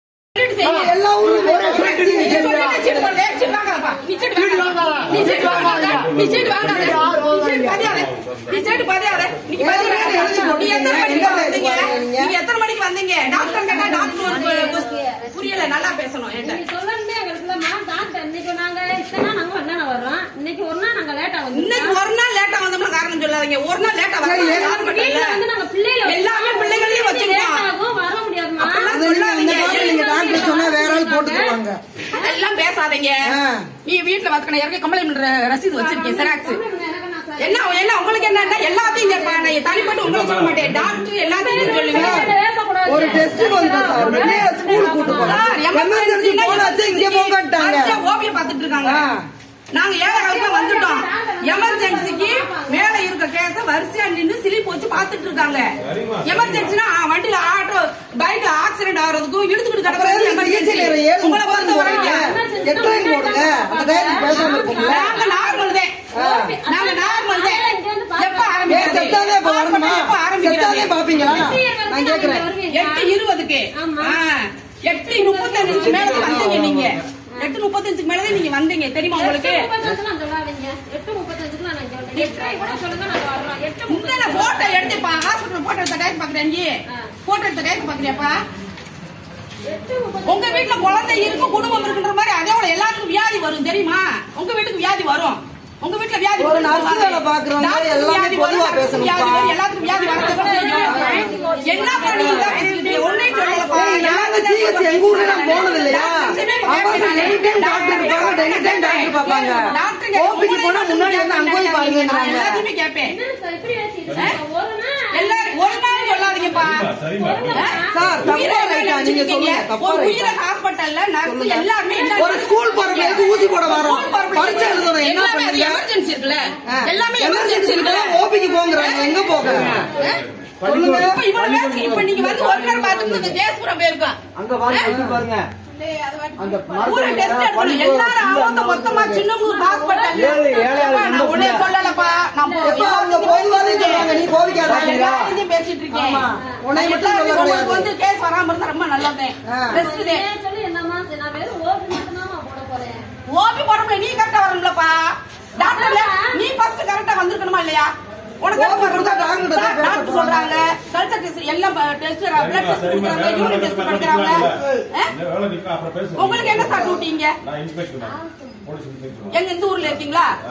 தாமதமாக வந்த ஊழியர்களை பொதுமக்கள் ஒன்றிணைந்து கேள்வி எழுப்பியதால் பரபரப்பு
சின்னமனூர் மருத்துவமனையில் தாமதமாக வந்த ஊழியர்களை  சரியான நேரத்திற்கு வந்து ஓபிச்சிட்டை வழங்க வேண்டும் என பொதுமக்கள் ஒன்றிணைந்து கேள்வி எழுப்பியதால் பரபரப்பு